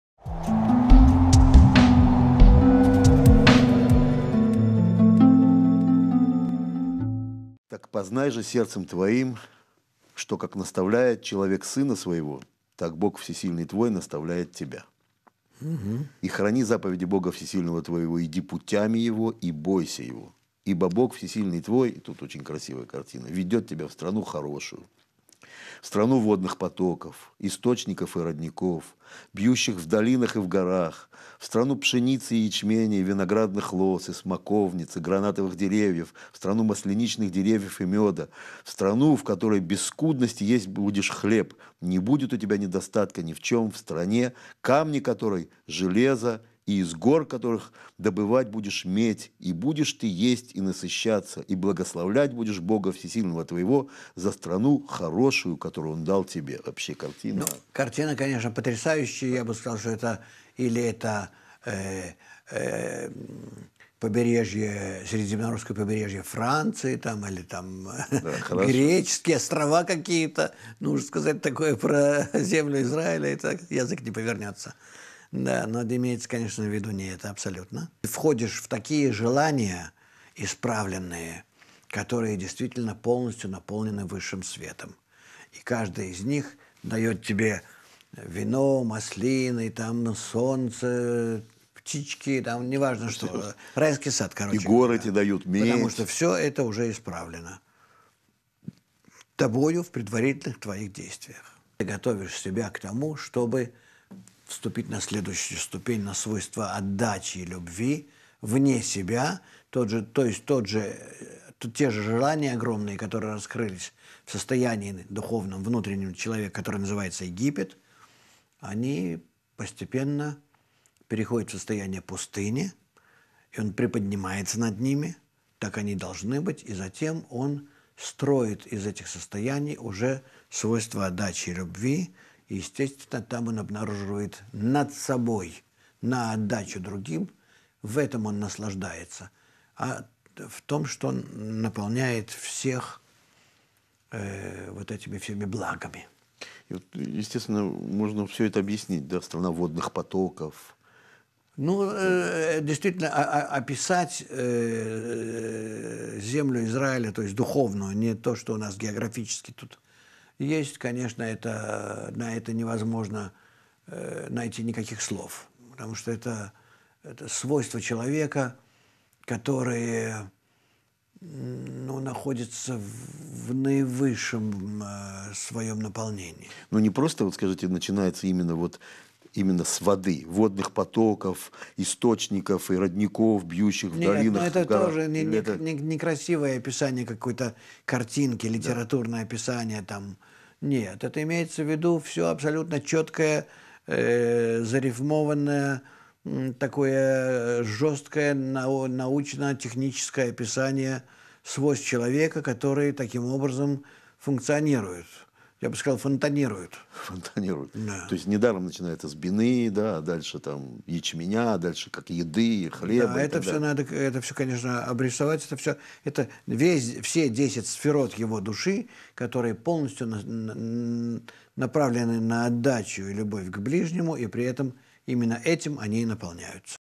Познай сердцем. Экев, передача 3
Чтобы описать духовную, а не географическую землю Израиля (желание к отдаче и любви), невозможно найти никаких слов. Потому что это свойства человека, который находится в наивысшем своем духовном наполнении.